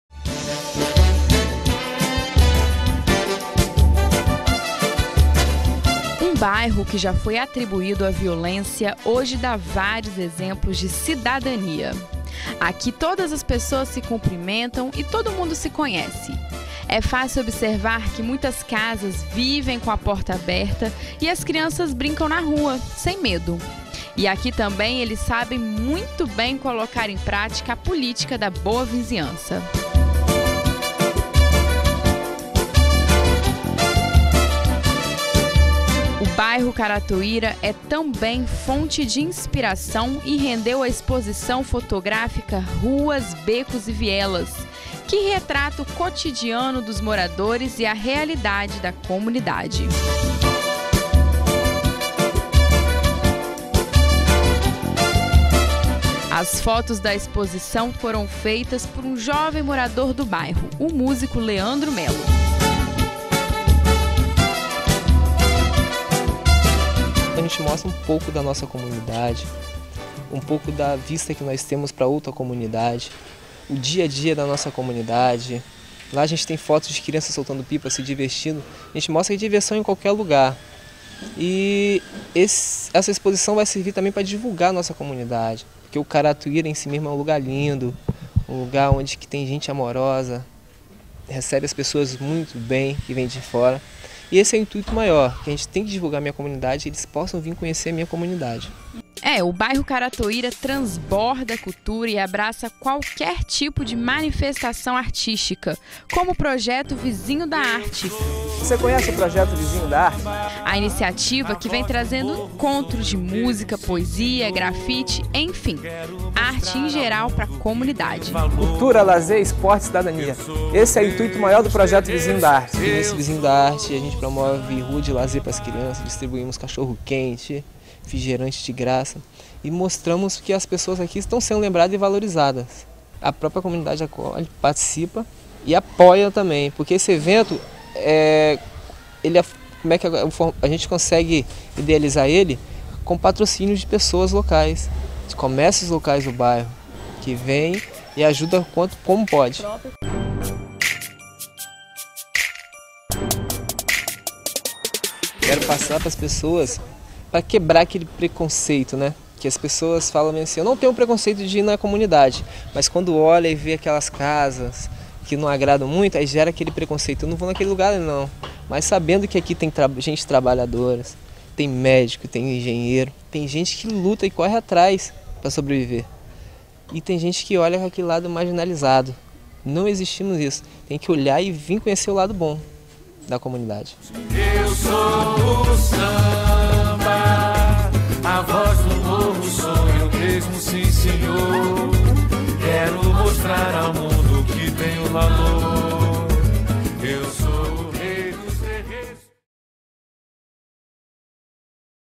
Leandro Mello - Músico
Vc confere agora o trabalho do músico Leandro Mello, nascido e criado no morro do Caratoíra, em Vitória, no Espírito Santo, começou sua vida nas rodas de samba do bairro. Hoje seu foco é a música e desenvolver projetos que valorizem os artistas do bairro, os talentos da comunidade.